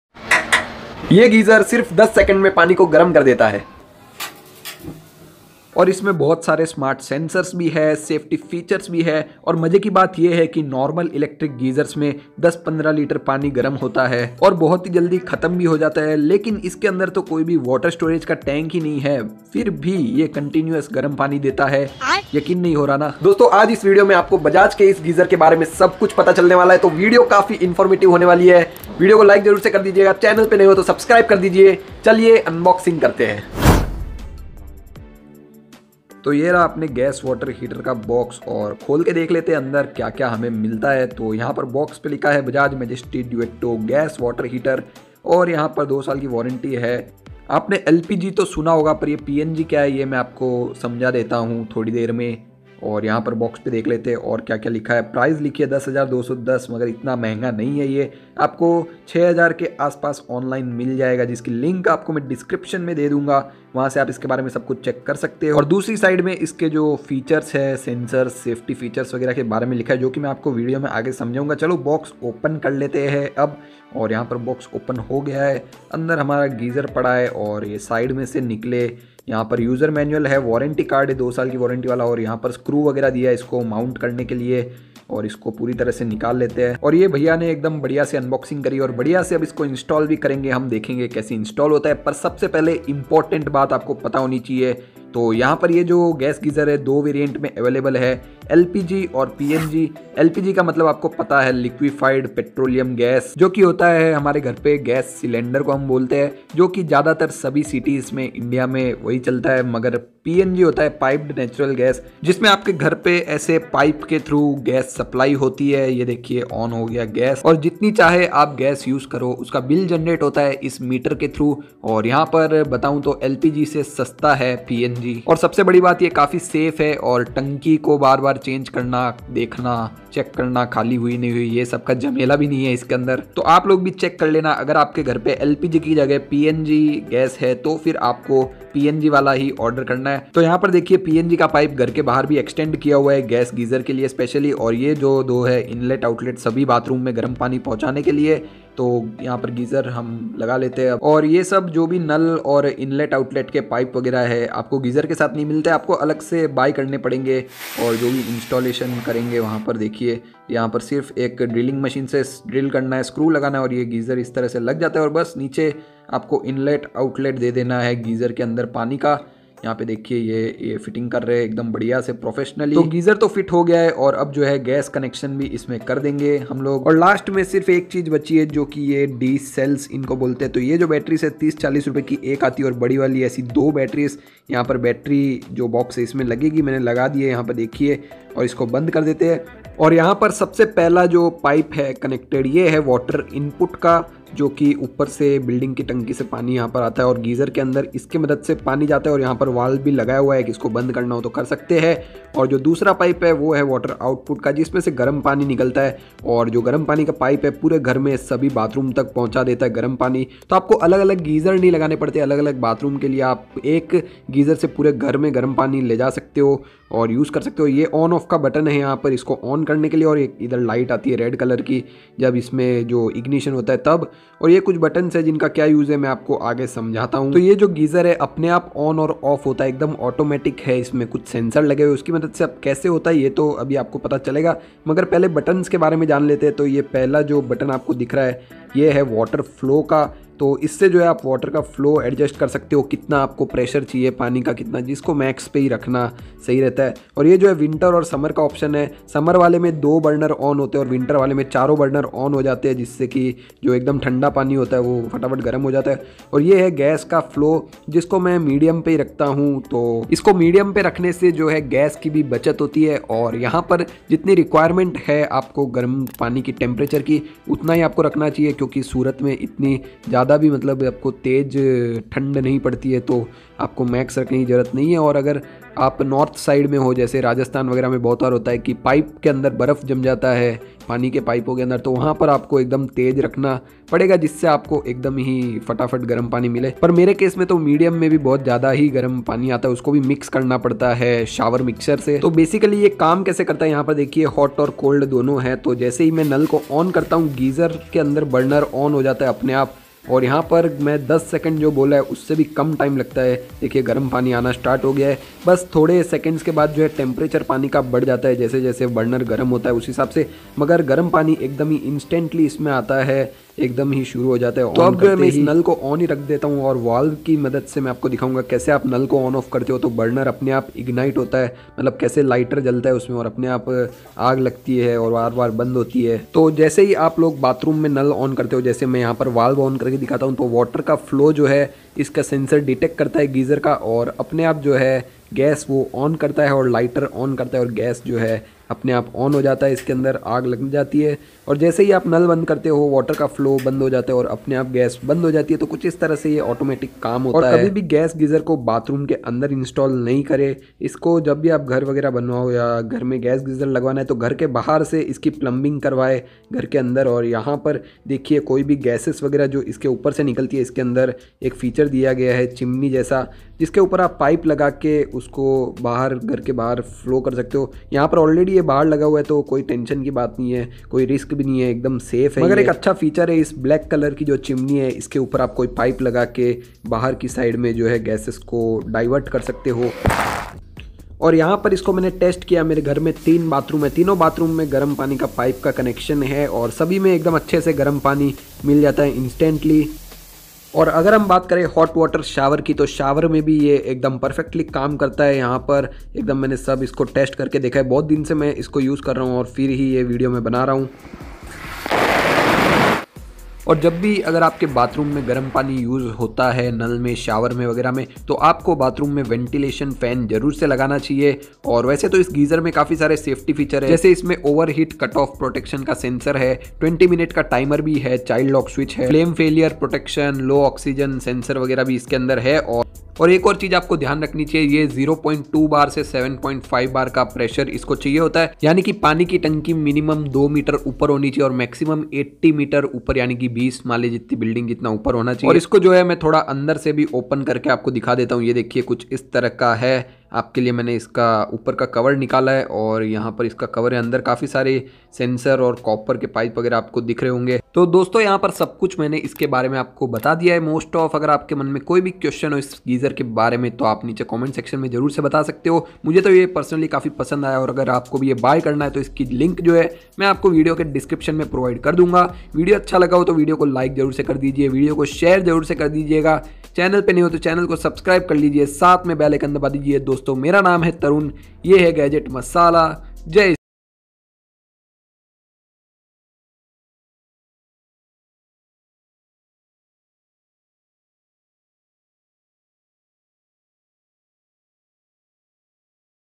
New Instant Water Heater | Bajaj Gas Geyser Unboxing & Review
Best New Instant Water Heater | Bajaj Gas Geyser Unboxing , Installation & Review
Bajaj Majesty Duetto Gas 6 Ltr Vertical Water Heater
LPG & PNG Geyser
Tech I'm using right now